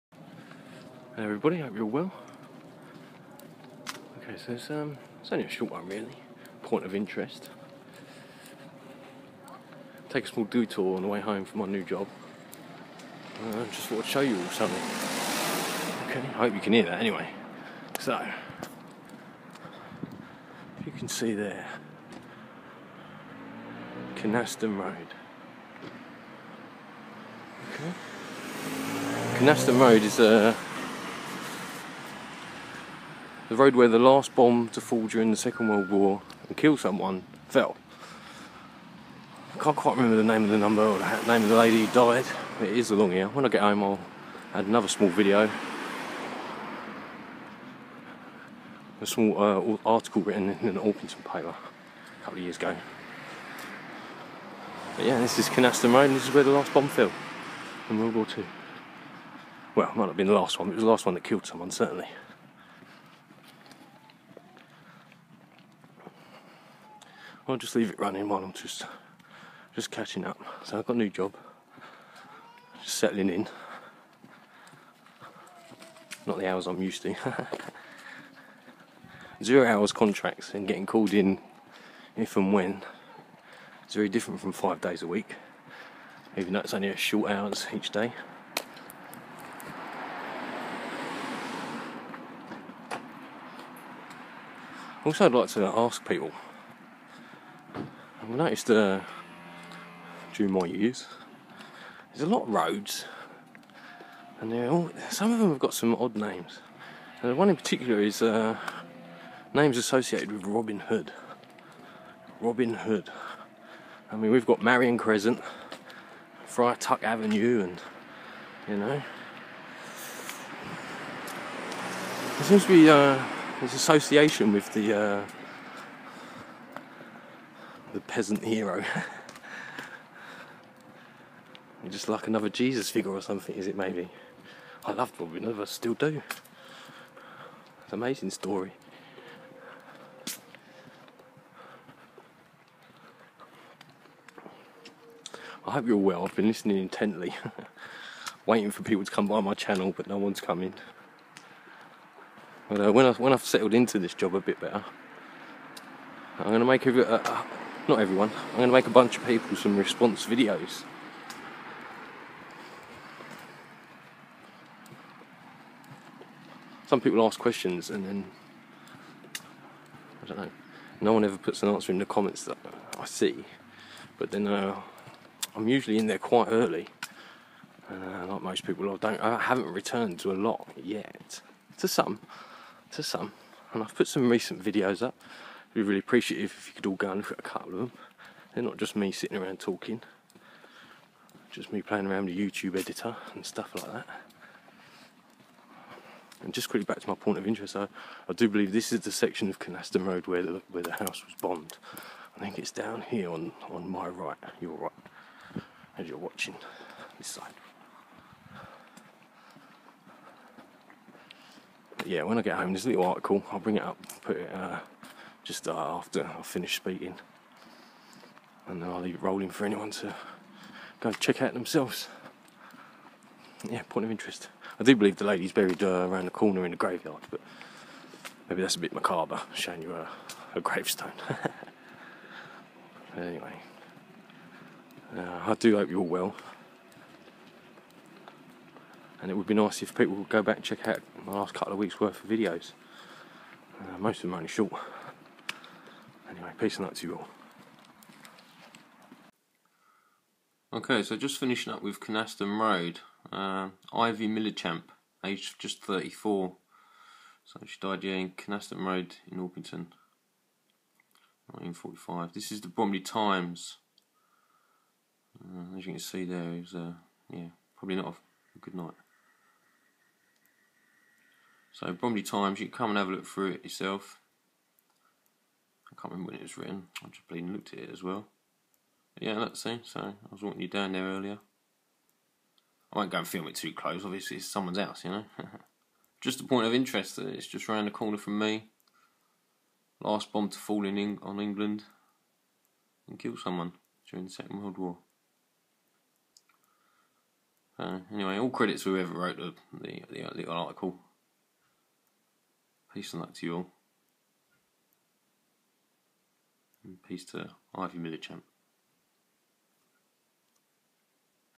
Flat Earth ... catching up on Kynaston Road
a small point of interest ... mentioned before by Cofo72 ... in my back yard, so to speak ... a small request to look back at some of my recent vids ... a small promise ... peace and luck
Ivy Millichamp - last person killed by a V2 rocket in 1945 - aged just 34  ...
there is also a book by local man Colin Philpott called A Place in History,credited with this finding 77 years after the fateful night Ivy Millichamp died ... with many other tidbits of information relating to many other of the more obscure subjects